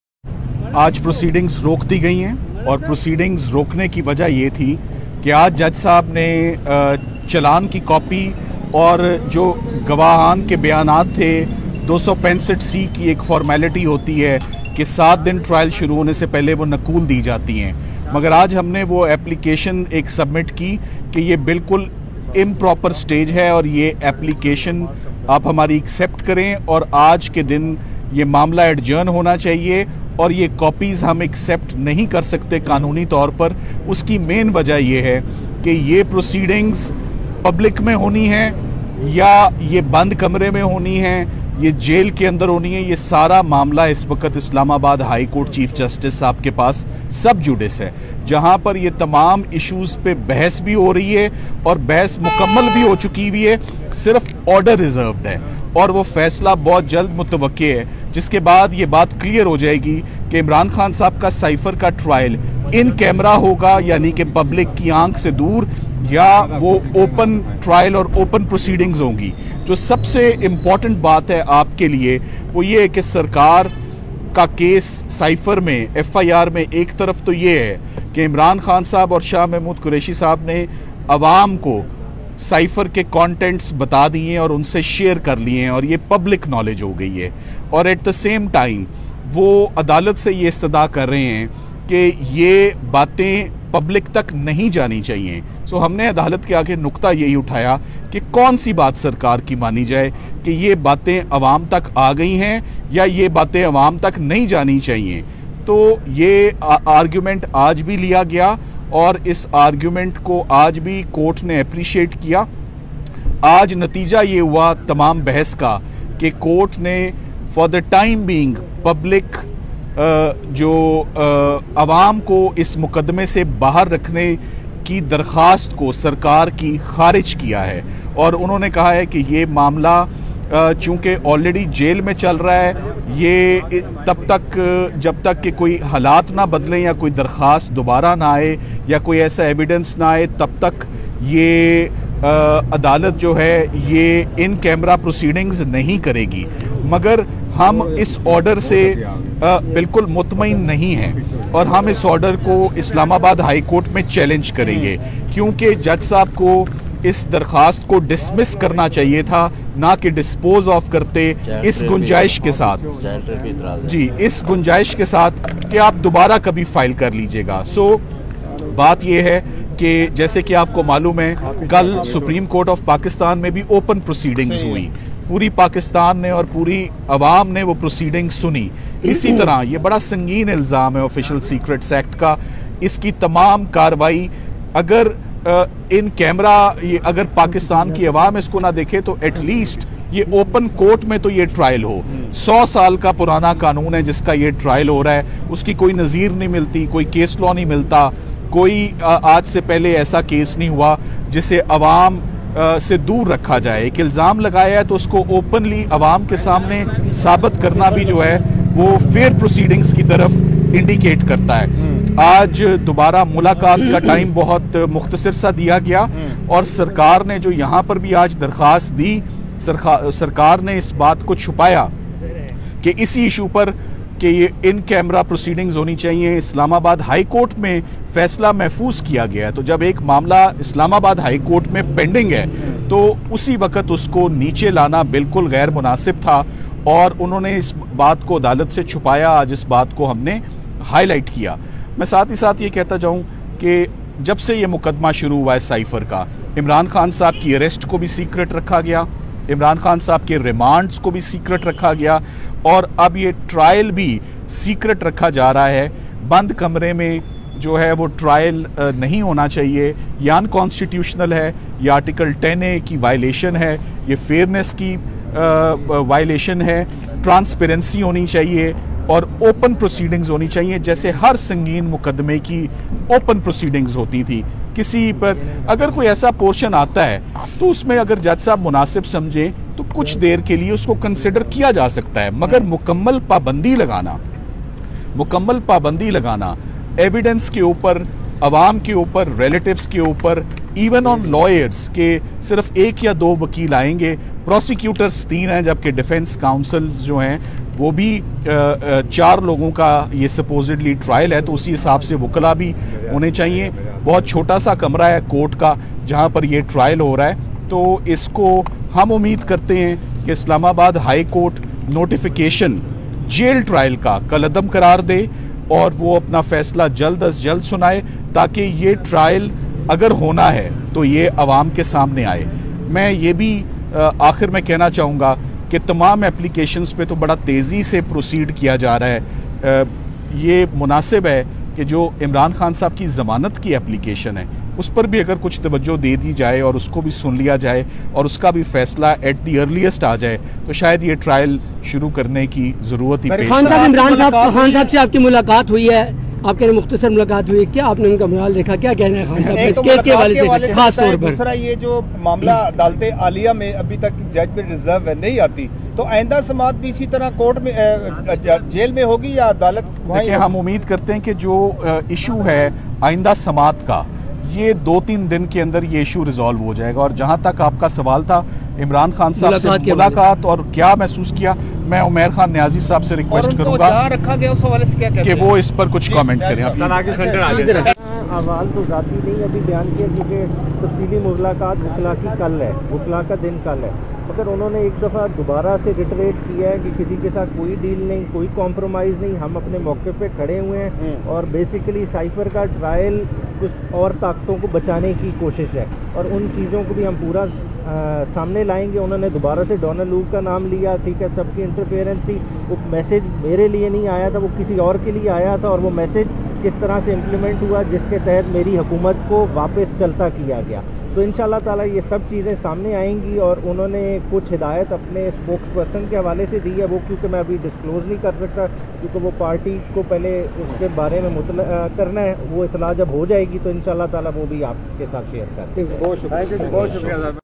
Chairman Imran Khan’s Lawyer ⁦‪Salman Safdar‬⁩ explains in detail the cypher case
Chairman Imran Khan’s Lawyer ⁦‪Salman Safdar‬⁩ explains in detail the cypher case, and how the secret jail trial violates all fundamental rights of both.